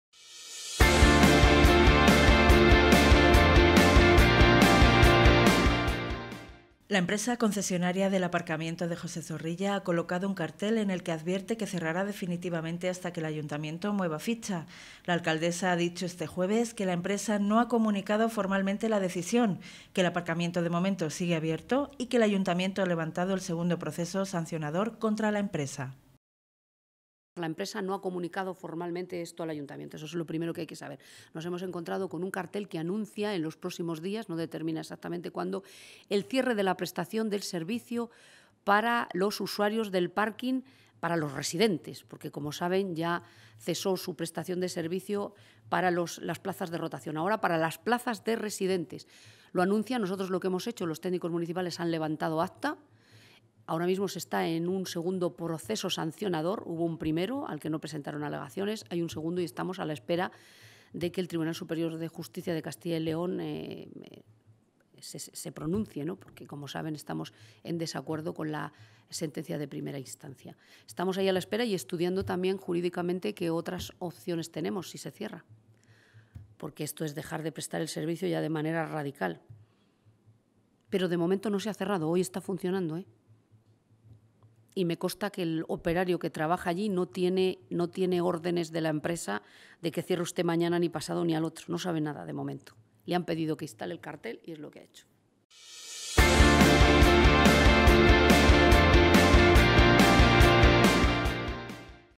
Clara Luquero reprocha el anuncio del cierre "radical" del parking de José Zorrilla
La empresa concesionaria del aparcamiento de José Zorrilla, Collosa-Pigsur, ha colocado un cartel en el que advierte que cerrará definitivamente, también para los clientes con plazas cedidas, hasta que el Ayuntamiento mueva ficha. La alcaldesa, Clara Luquero, ha dicho este jueves que la empresa no ha comunicado formalmente la decisión, que el aparcamiento de momento sigue abierto y que el Ayuntamiento ha levantado el segundo proceso sancionador contra la empresa.